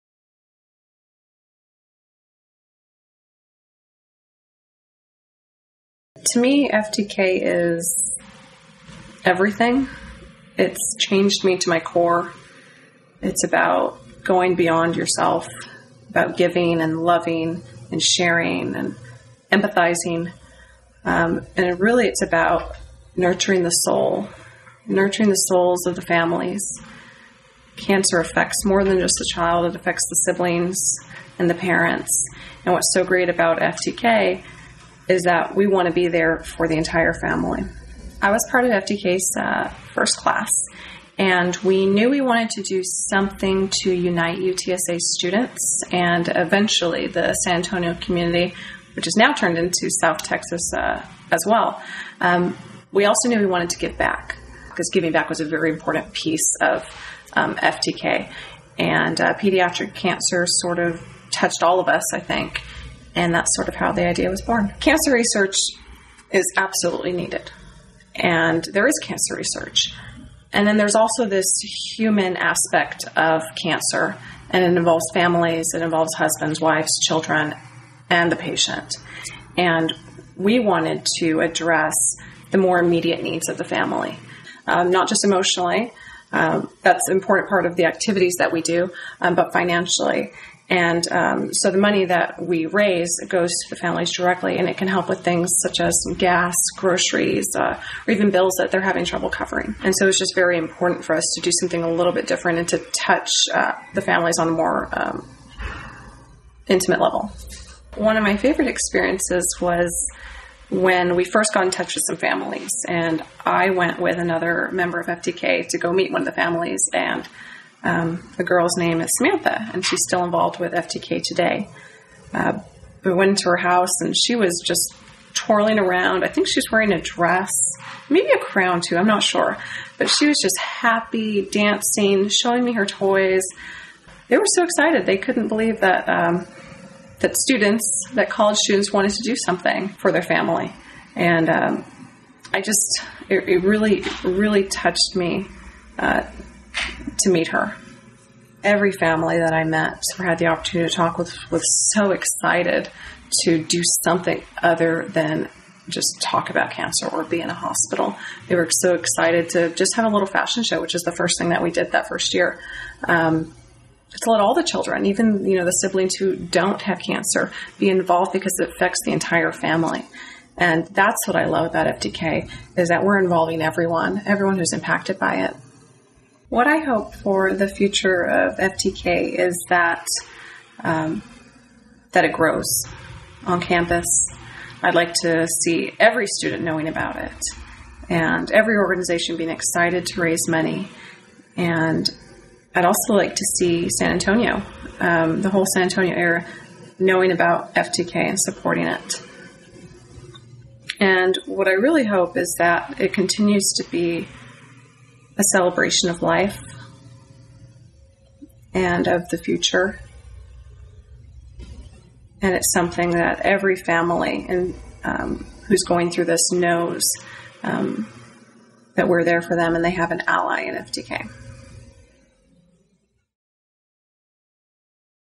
FTK: April's Story
What is FTK?  Simply, it's an organization that raises money and awareness for those affected by childhood cancer.  However, what we do has a lasting impact on those who help bring to fruition our yearlong efforts.  This is April's story.

If you can't make it, you can give through our Razoo account (all donations go directly to the families):